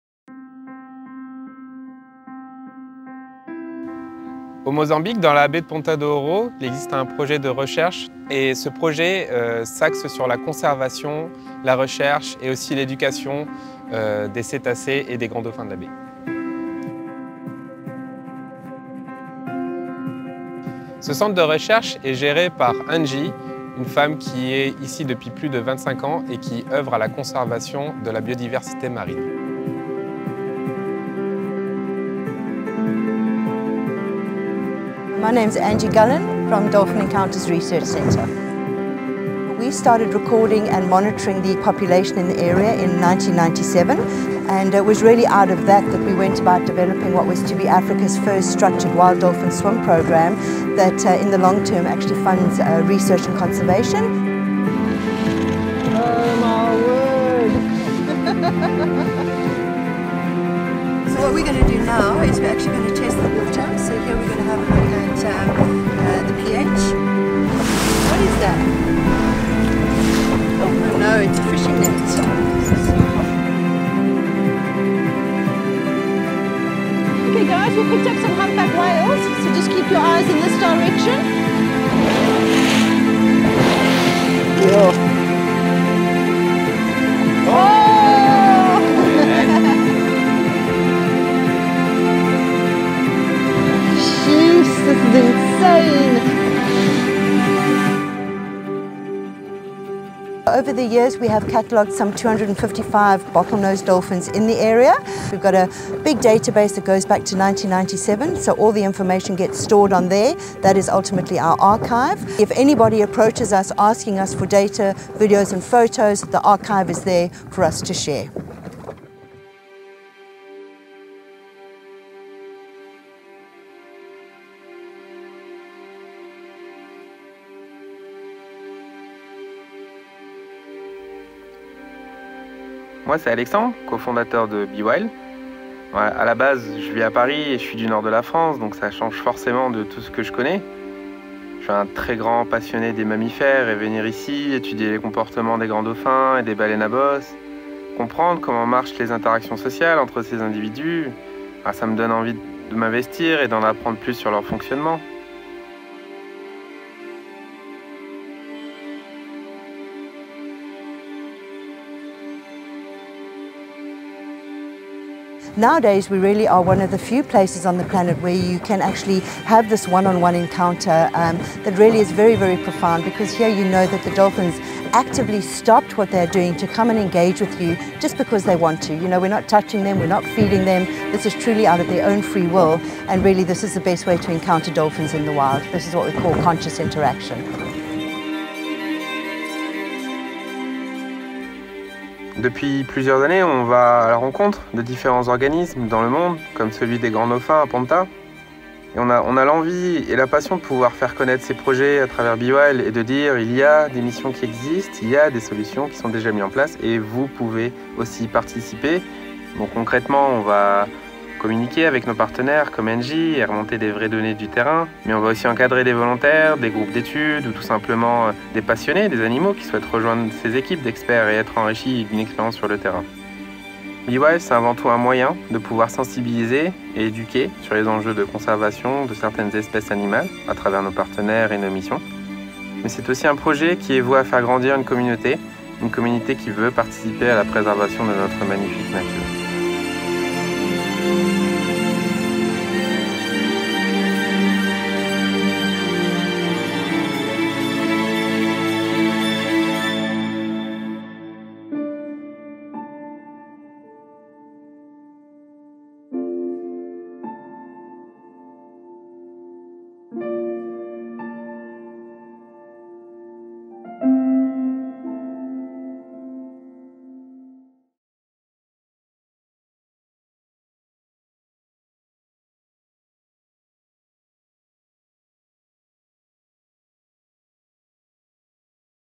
PEOPLE OF THE SEA - Notre mission d'écovolontariat au Mozambique
Nous sommes hyper fiers de vous montrer ce mini-documentaire sur notre mission au Mozambique !

Nous avions à cœur de sortir ce documentaire ce 3 Mars, Journée internationale de la vie sauvage  🍃
Une manière pour nous de rendre hommage à Mère Nature et à toutes ces merveilles qu'elle nous réserve.

L’occasion de vous présenter plus concrètement un de nos partenaires au Mozambique  @dolphincareafrica  . Cette association œuvre à la recherche et la conservation des grands dauphins sauvages et des baleines à bosses 🐋

Des interactions et des moments rares filmés au plus proche de la Nature, situé dans la magnifique baie de Ponta Do Ouro et au pied de l’Océan Indien… 🌊😍

😁 Une production 100% Bwild

🎥 La réalisation et le montage par le talentueux  @rudiwild1278

🙏 Et un projet qui a permis de voir le jour grâce à notre sponsor  @hiway8034

Alex, l'un de nos cofondateurs, avait depuis longtemps ce rêve : réaliser un documentaire au plus proche de la Nature, dans un environnement sauvage et avec des spécialistes de la conservation d’espèces animales. 🐬

Ce documentaire, c’est avant tout le fruit de plusieurs rencontres :

💪 Avec Laure, notre cofondatrice, qui a la passion et la volonté de toujours dénicher les projets de conservation éthiques et ayant un impact.

🤝 Avec l’équipe de recherche d’Angie, qui œuvre au quotidien pour la préservation des grands dauphins indo-pacifiques dans le parc national de Maputo.

📹 Avec Rudi aka  @rudiwild1278  , un jeune et talentueux réalisateur, spécialiste des grands espaces sauvages. Il a su capter, caméra à la main, la vision pour raconter l’histoire d’Angie et des grands dauphins

Un tournage de 7 jours, riche en émotions.
7 jours intenses et où chaque journée eut son lot de surprises.
3 jours de doute pour commencer sans pouvoir aller en mer, à cause d'une tempête...

... Mais 7 jours pendant lesquels la Nature nous a offert ce qu’elle a de plus beau et de plus mystérieux ✨
🏊‍♂️ Des nages circulaires avec les grands dauphins,
🐳 Des acrobaties de baleines à bosse,
🐬 Des migrations massives de dauphins à long bec…
🙏 Des intéractions intimes et dans une approche respectueuses de ces magnifiques espèces marines.

Un énorme merci à toute l’équipe de Bwild, de Dolphin Encountours Research Center et de notre sponsor Hiway, sans qui ce documentaire n’aurait pas été possible.

Nous espérons que vous apprécierez la vidéo autant que nous avons eu le plaisir de la réaliser 😊

Nous avons à cœur de raconter d’autres histoires dans le futur avec nos partenaires sur le terrain. Montrer cette connexion spéciale et vitale entre l’Homme et la Nature, en harmonie pour la préservation de notre magnifique planète.

A très vite pour de prochaines aventures ! 😊

Et si ça vous a donné envie de partir, on vous laisse le lien de notre